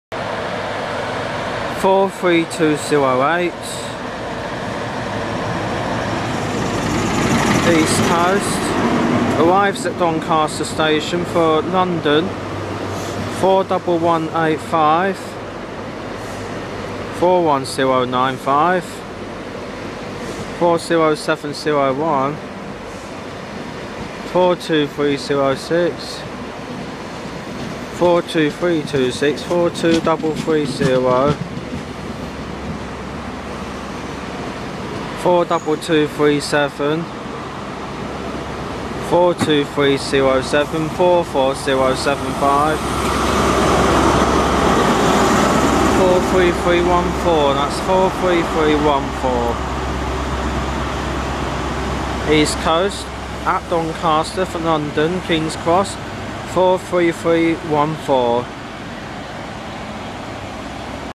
East Coast 43208 and 43314
Arrives into Doncaster for London Kings Cross